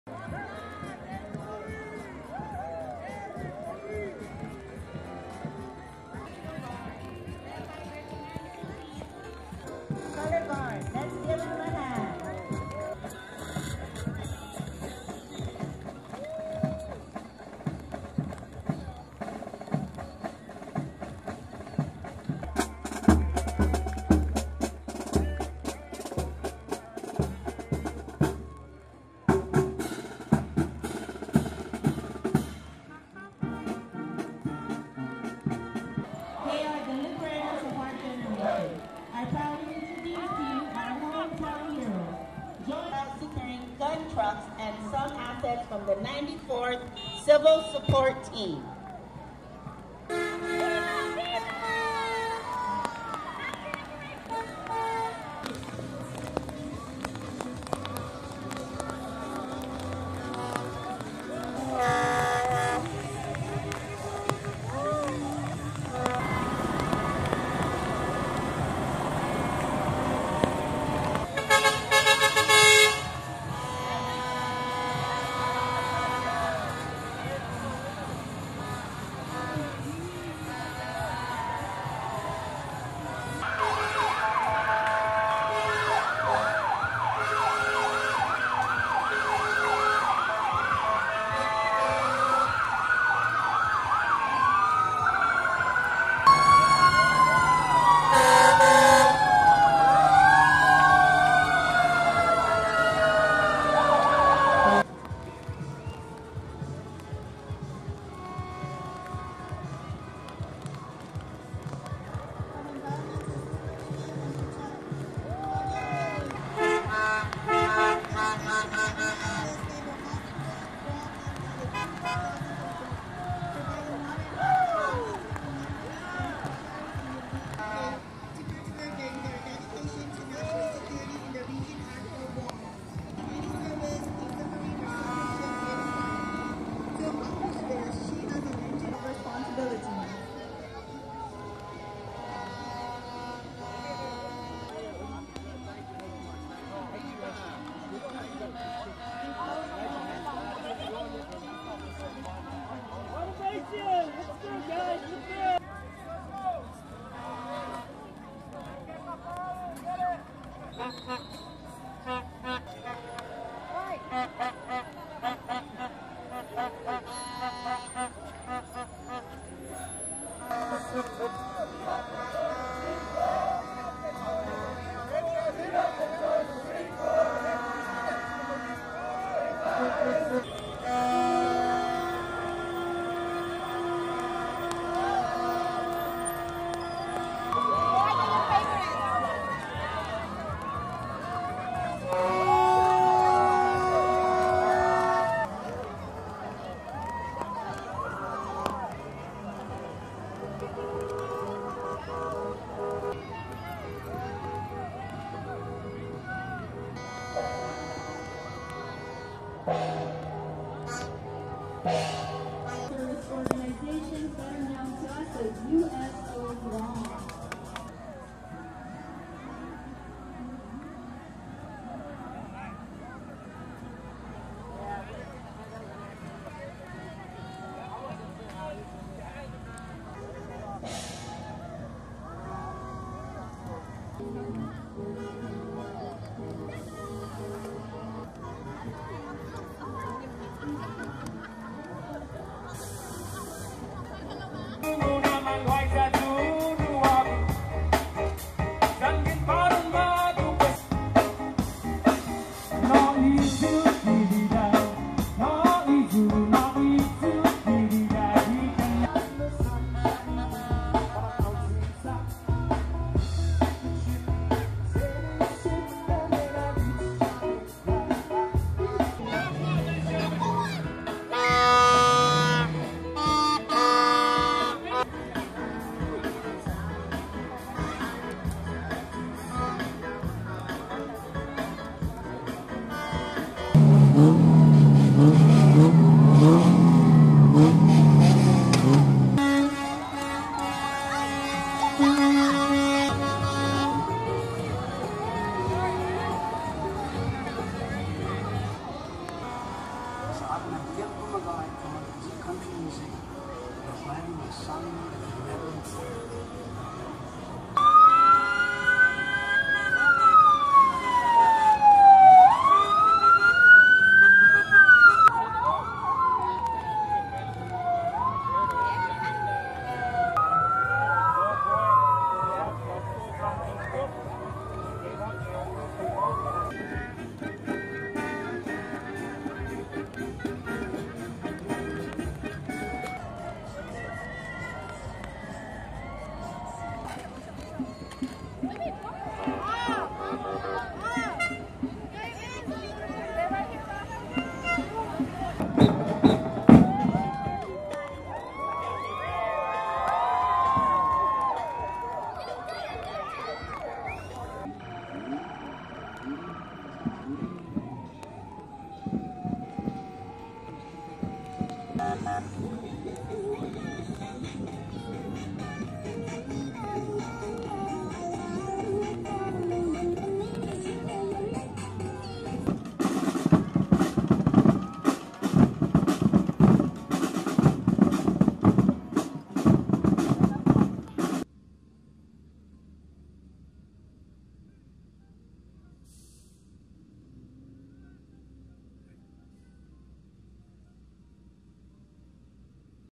Liberation Day Parade 2022 | Guam b-roll
This video depicts b-roll footage of the Liberation Day Parade on Guam, July 21, 2022. This parade marked the 78th annual commemoration of Guam’s liberation by the U.S. military forces on July 21, 1944, which ended the Japanese occupation that began in 1941. (US. Air Force video by SSgt. Ryan Brooks)

Liberation Day 2022 b-roll
GUAM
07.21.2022

Film Credits: Video by Staff Sgt. Ryan Brooks
36th Wing Public Affairs